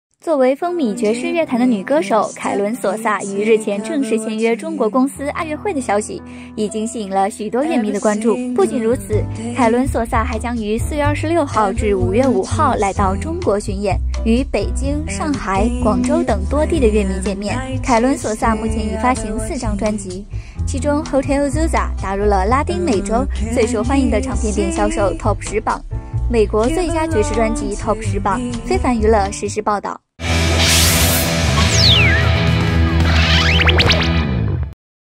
【2年前】爵士情調女王凱倫·索薩啟動中國巡演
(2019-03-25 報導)

作為風靡爵士樂壇的女歌手，凱倫·索薩（Karen Souza）于日前正式簽約中國公司愛樂匯的消息，已經吸引了許多樂迷的關注。不僅如此，凱倫·索薩還即將于4月26日至5月5日來到中國巡演，與北京、上海、廣州等多地的樂迷見面。